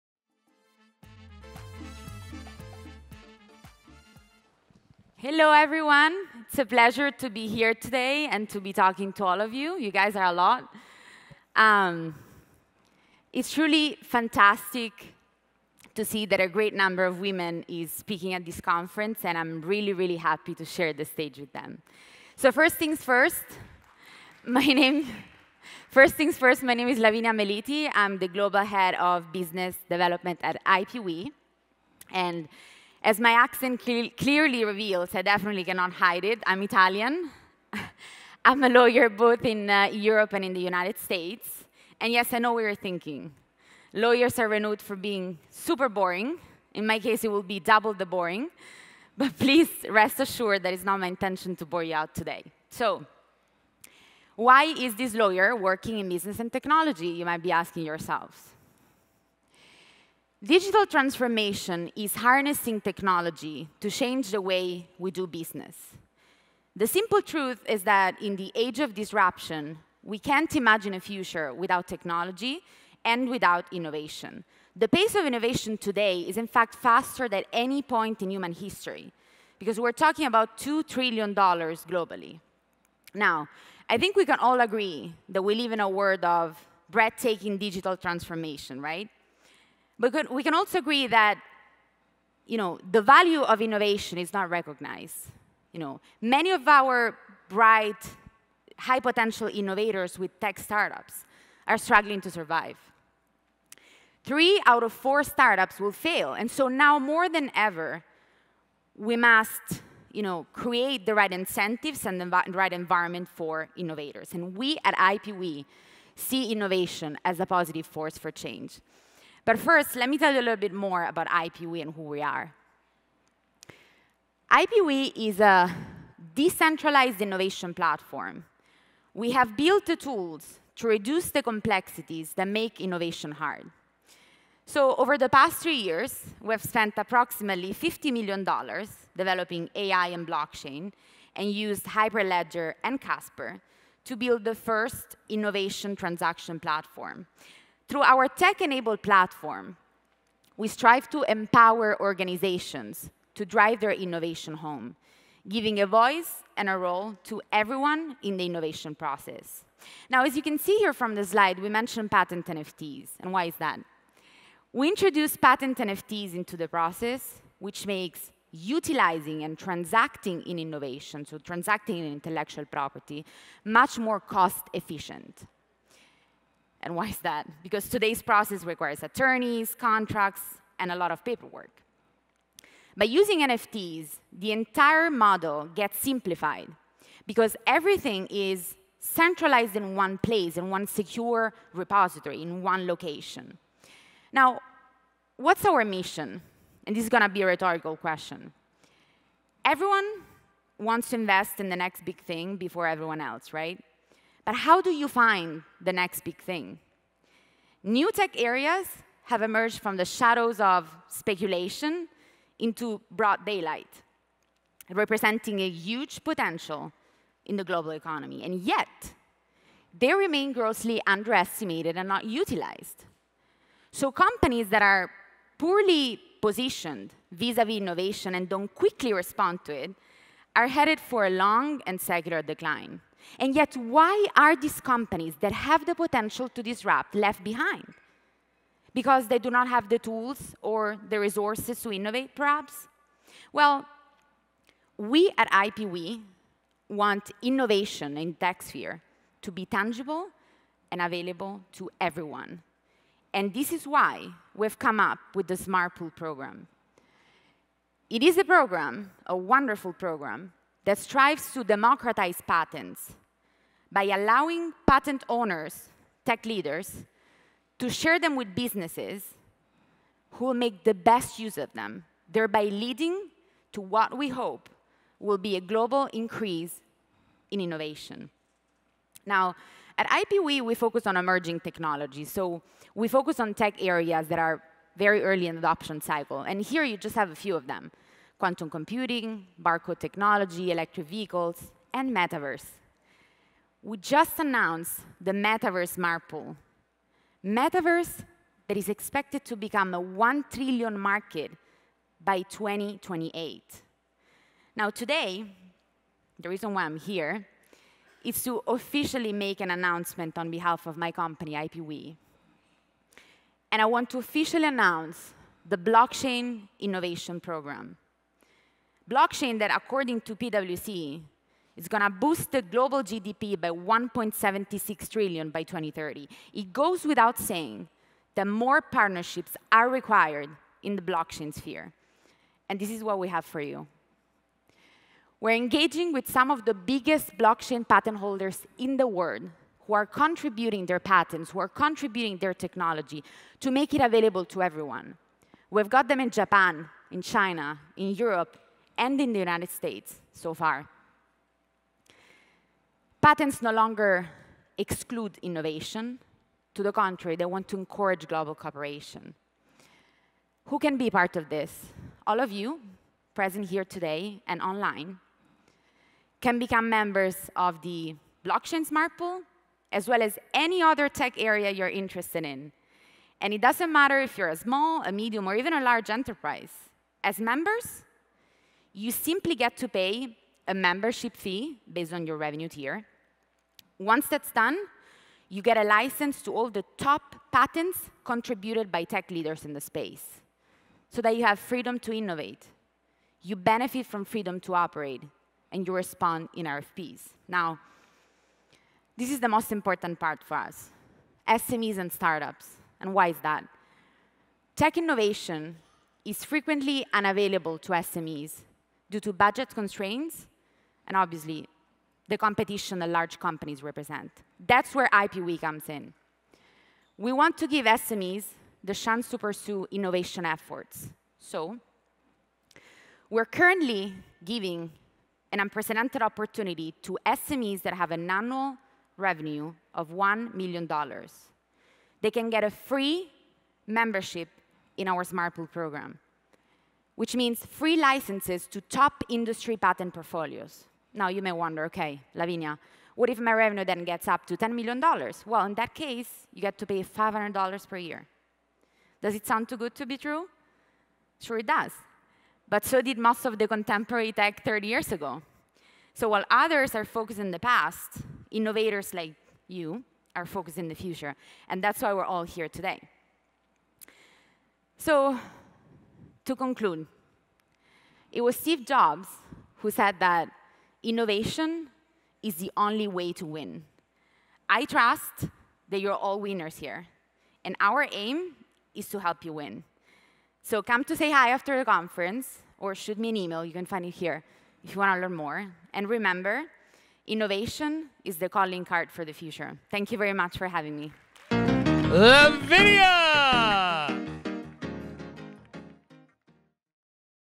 Lavinia Meliti walks us through the blockchain-enabled innovation platform | TNABC 2022
The North American Bitcoin Conference 2022
The Blockchain-enabled Innovation Platform by Lavinia Meliti